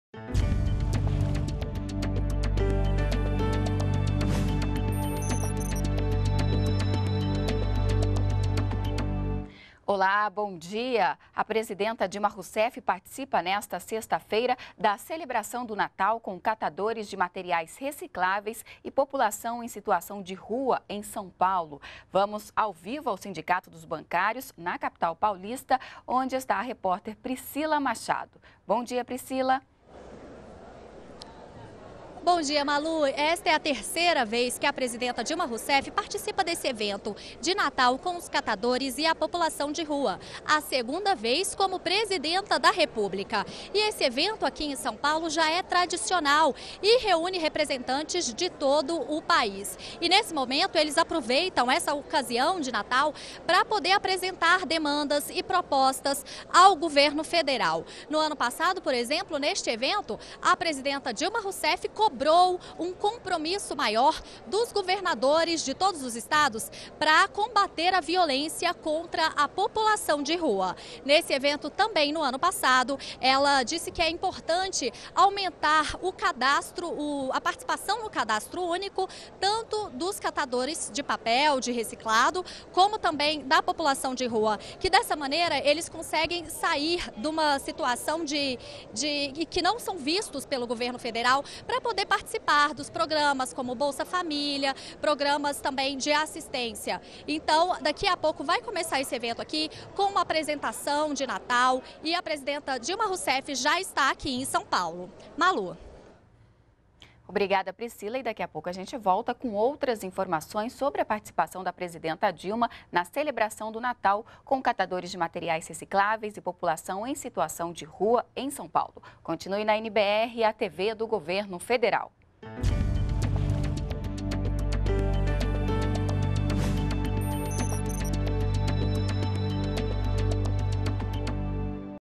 Dilma Rousseff participa da celebração de Natal com catadores de materiais recicláveis
ATIVIDADES DA PRESIDENTA - 21.12.12: Dilma Rousseff participa, nesta sexta-feira (21), da cerimônia de celebração do Natal dos catadores e da população em situação de rua, em São Paulo. Essa é a terceira vez que a presidenta participa do evento, que reúne representantes de todo o país.